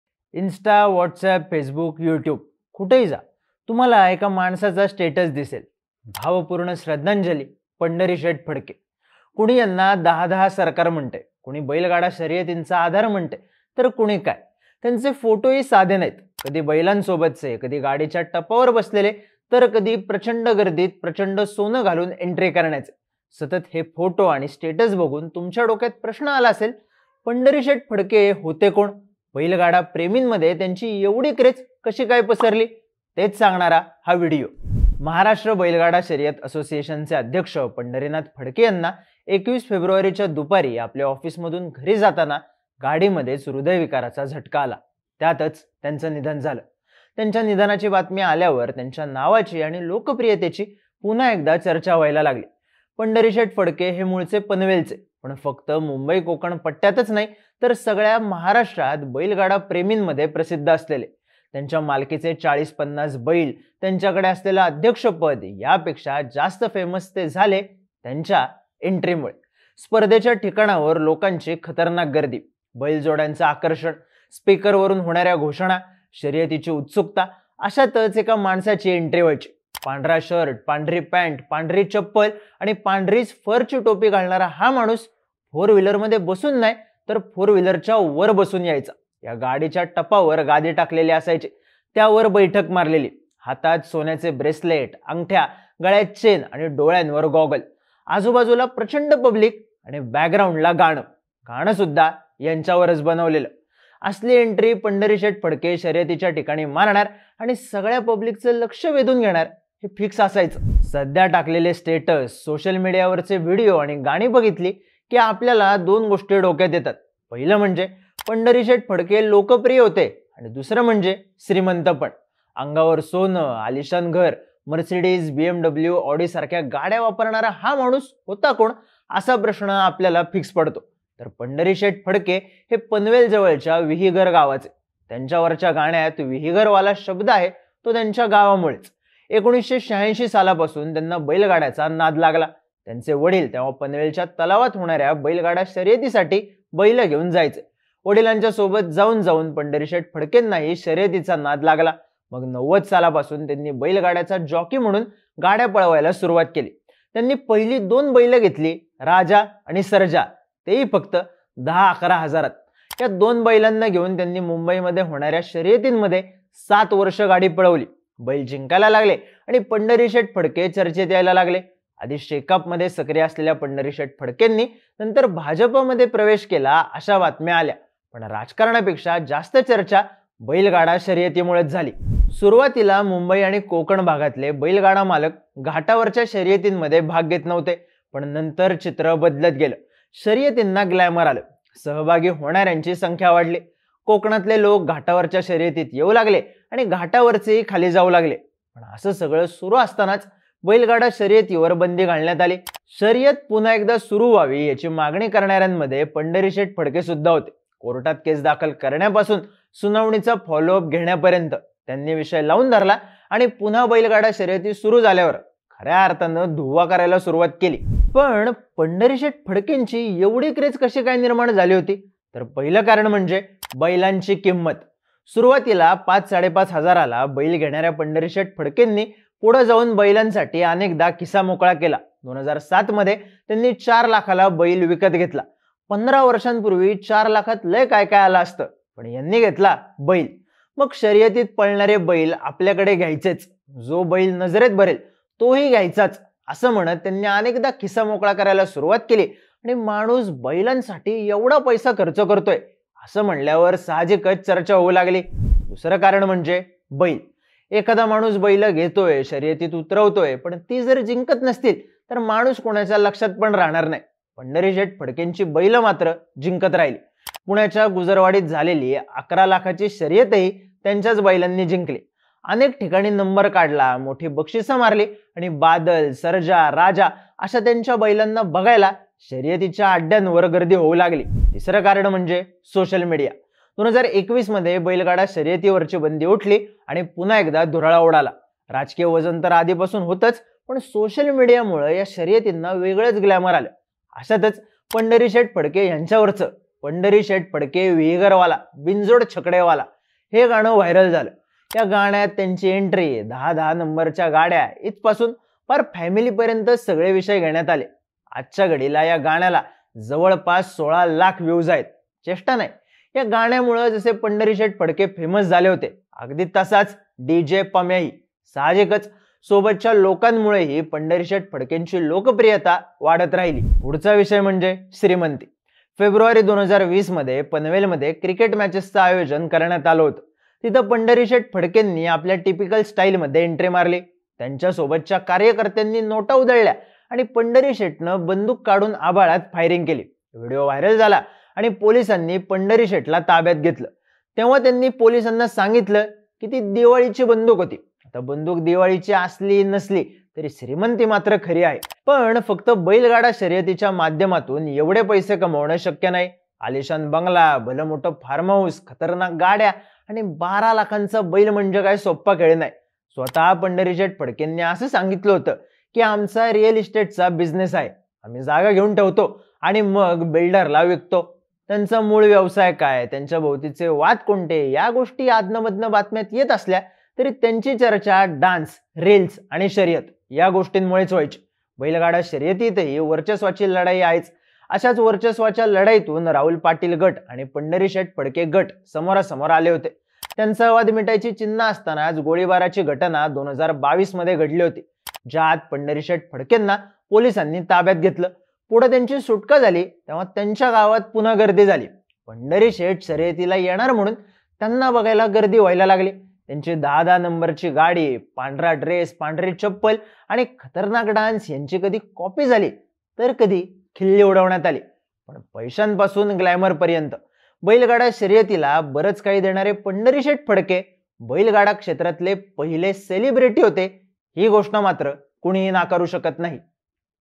Pandhari Sheth Phadke Death: बातम्यांपासून Social Media पर्यंत चर्चेत असलेले पंढरीशेठ फडके कोण होते?
इन्स्टा, व्हाट्सअप, फेसबुक, युट्युब कुठंही जा, तुम्हाला एका माणसाचा स्टेटस दिसेल, भावपूर्ण श्रद्धांजली, पंढरीशेठ फडके. कुणी यांना दहादहा सरकार म्हणतंय, कुणी बैलगाडा शर्यतींचा आधार म्हणतंय, तर कुणी काय. त्यांचे फोटोही साधे नाहीत, कधी बैलांसोबतचे, कधी गाडीच्या टपावर बसलेले, तर कधी प्रचंड गर्दीत प्रचंड सोनं घालून एंट्री करण्याचे.

महाराष्ट्र बैलगाडा शर्यत असोसिएशनचे अध्यक्ष पंढरीनाथ फडके यांना २१ फेब्रुवारीच्या दुपारी आपल्या ऑफिसमधून घरी जाताना गाडीमध्येच हृदयविकाराचा झटका आला. त्यातच त्यांचं निधन झालं. त्यांच्या निधनाची बातमी आल्यावर, त्यांच्या नावाची आणि लोकप्रियतेची पुन्हा चर्चा व्हायला लागली. पण पंढरीशेठ फडके होते कोण ? बैलगाडा प्रेमींमध्ये त्यांची एवढी क्रेझ कशी काय ? तेच सांगणारा हा व्हिडीओ.